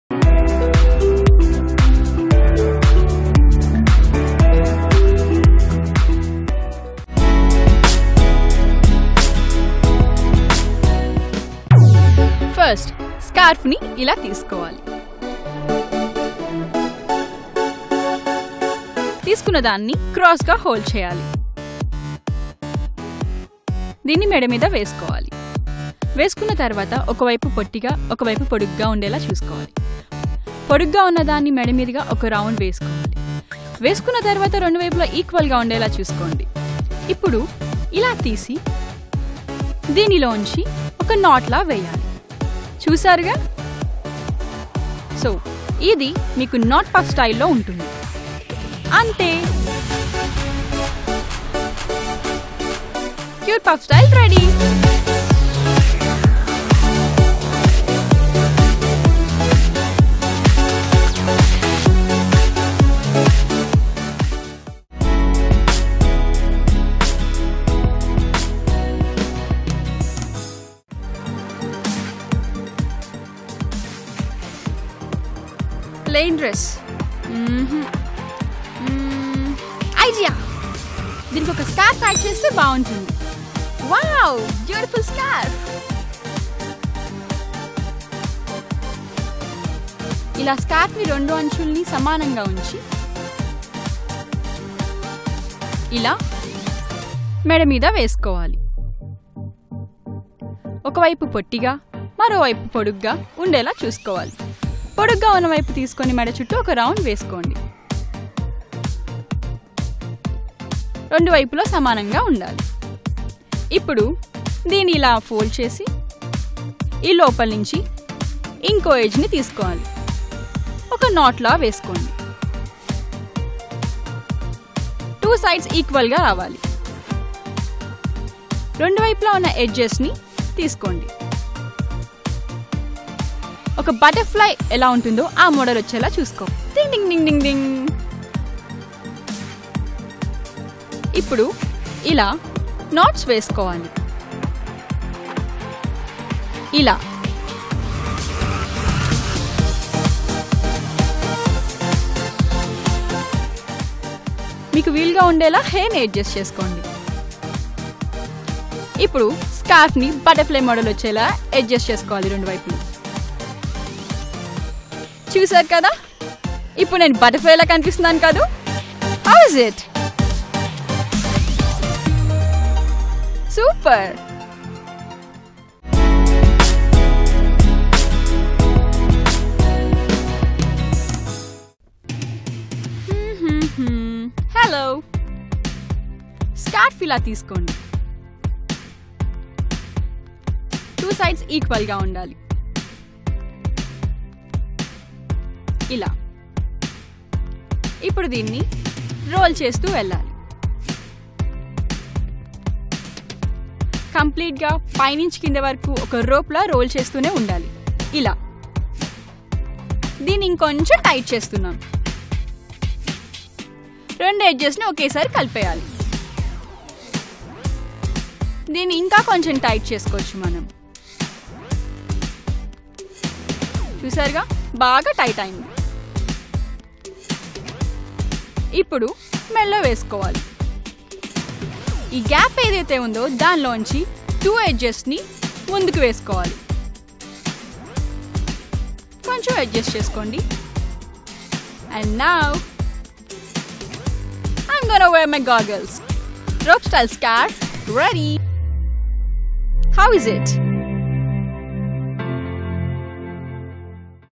Different Styles in Wearing Scarf for Girls | Fashion & Beauty by ETV
How to wear a scarf around your neck in 5 different ways.Wear scarf in the latest trending styles and make your outfit interesting. Try it. You would love it…..
Latest trends in fashion and styling alongside makeup and beauty tips, and the unmissable grooming and makeover ideas